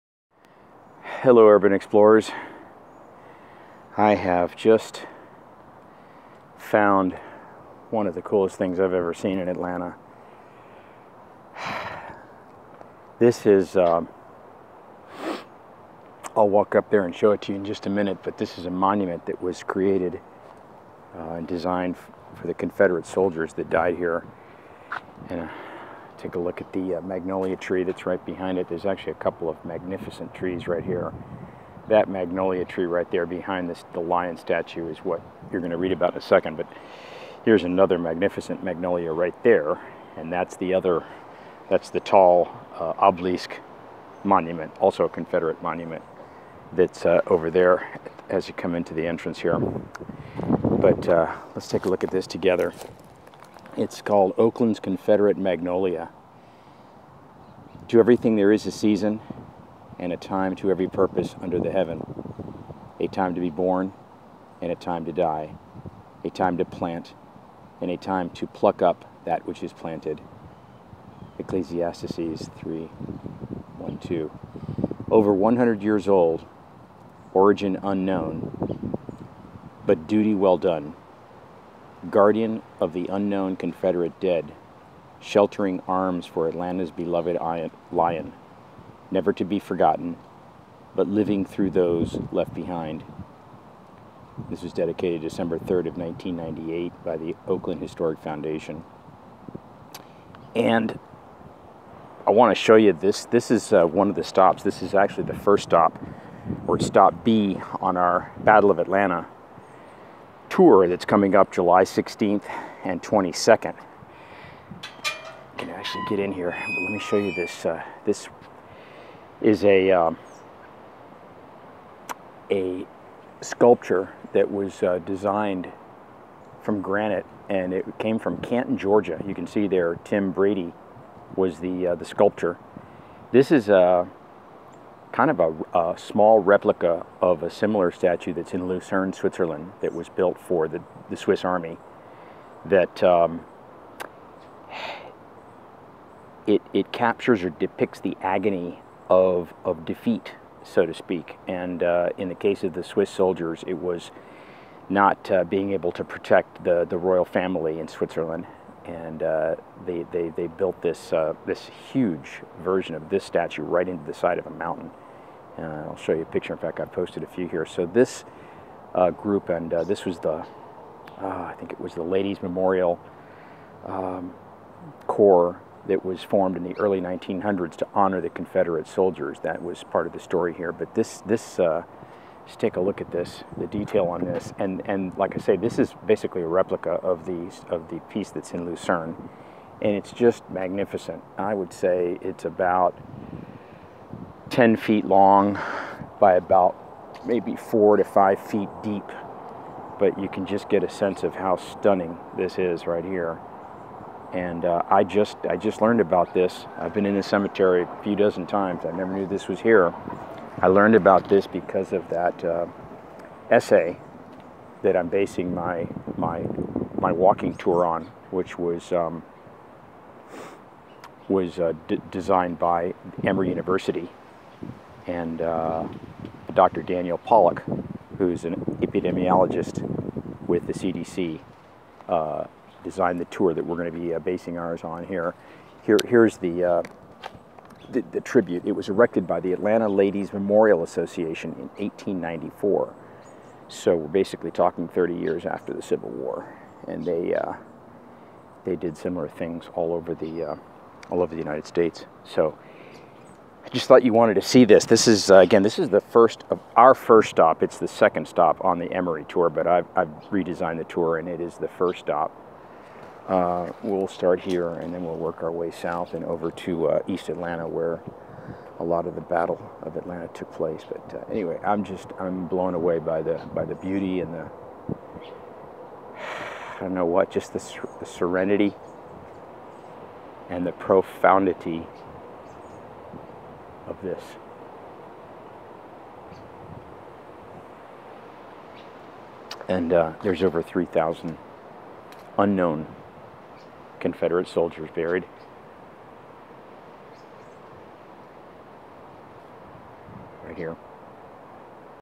The Atlanta Lion in Oakland Cemetery
Video by: ATLsherpa

In preparation for our upcoming Battle of Atlanta: A Walking Tour of History and Remembrance on July 16 & 22, I am doing some research on what we will be seeing at the various stops here on the Urban Explorers of Atlanta FB page. I am, in a word, awestruck by what I am learning. For this reason, I am going to start sharing some of the more interesting stories. Hope you can make one of these tours. This story is from our "Stop A,", which is "Stop #2" on the Emory tour...

The Ladies Memorial Association* erected two large monuments (in Historic Oakland Cemetery) that mark the space set aside for the (civil) war dead: a sixty-five-foot-tall obelisk of Stone Mountain granite and a six-foot-high statue of a grieving lion carved from a fifteen-ton piece of Tate, Georgia, marble and modeled after the renowned Lion of Lucerne in Switzerland.

The Lion of Atlanta, like the much larger Swiss statue, symbolizes the agonizing death struggle of an armed force in defeat, overwhelmed by its opponent but honored for bravery. In Atlanta, the monument commemorates the (3,000) unknown Confederate dead, whose remains are buried in the surrounding plot; in Lucerne, the carving that Mark Twain called "the most mournful and moving piece of stone in the world" is dedicated to the approximately 600 Swiss Guards who died while unsuccessfully defending the French royal family in August 1792, during the French Revolution. In each instance, a dying lion, mortally wounded by a lance driven through its back, clutches an emblem of the lost cause for which it fought: a Confederate battle flag in Atlanta and the French fleur-de-lis in Lucerne.

The Lucerne statue was sculpted in 1820–1821 with the support of Swiss aristocrats seeking the backing of Bourbon monarchs who had been restored to the French throne. Following the instructions of the Ladies Memorial Association, Thomas M. Brady of Canton, Georgia sculpted the Atlanta version, completing the statue in 1894. It was dedicated on April 26 of that year.
Twenty five years later, sculptor Daniel Chester French used white marble quarried in Tate, Georgia, for his colossal Abraham Lincoln Memorial statue in Washington, DC. Thus, marble from the same small town in Pickens County went to sculpt a memorial to thousands of unknown Confederate dead as well as a monument to the war's best known casualty.

* Ladies Memorial Associations are organizations of women that sprang up all over the American South in the years after the American Civil War. Typically, these were organizations by and for wealthy white women, whose goal was to inter or re-inter the bodies of Confederate soldiers and to raise monuments in their honor. Their immediate goal, of providing decent burial for soldiers, was joined with the desire to commemorate the sacrifices of Southerners and to propagate the Lost Cause of the Confederacy. Between 1865 and 1900, these associations were a formidable force in Southern culture, establishing cemeteries and raising large monuments often in very conspicuous places, and helped unite white Southerners in an ideology at once therapeutic and political.

Source: Wikipedia
Photo: Matt Miller
Source: Daniel A. Pollock, MD, Southern Spaces